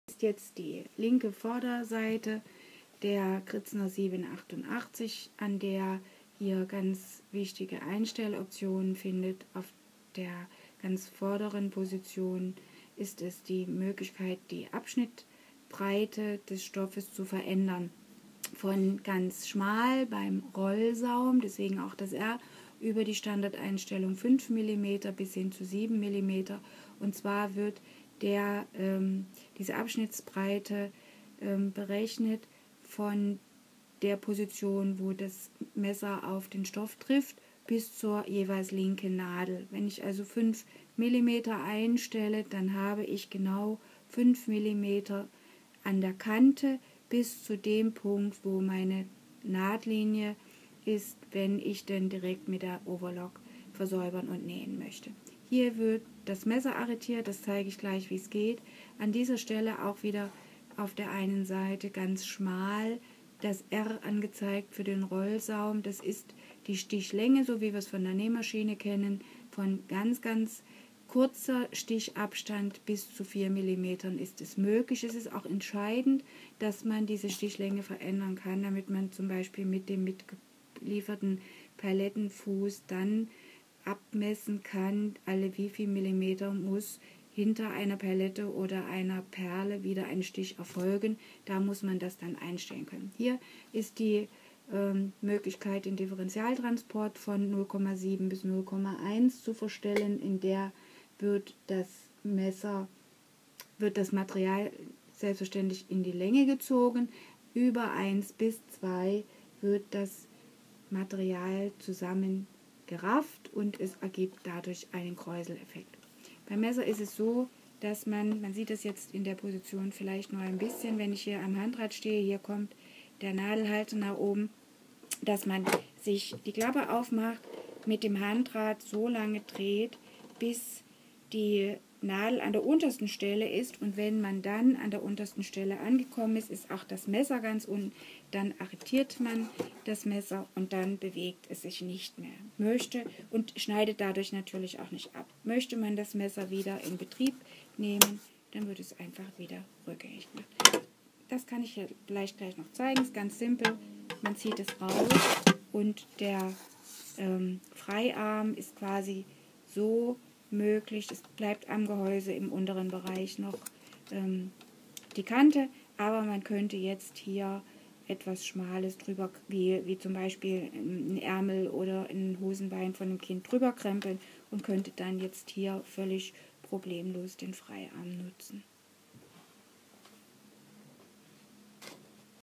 Bedienung der Gritzner 788
Was wie wozu? Das ist eine Kurzerklärung zur Maschine. Mehr Informationen gibt es zum Nähen mit der Overlock auf meiner